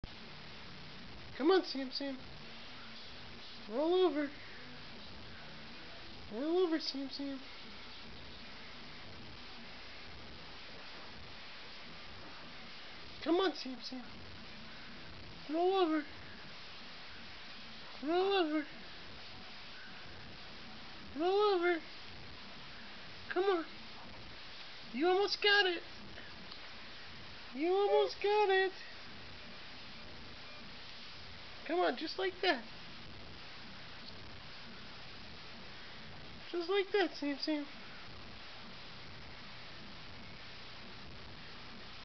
Sam considers rolling from back to tummy
With a wiggle of his booty and a kick of his feet, he lunges around, but falls back to his back.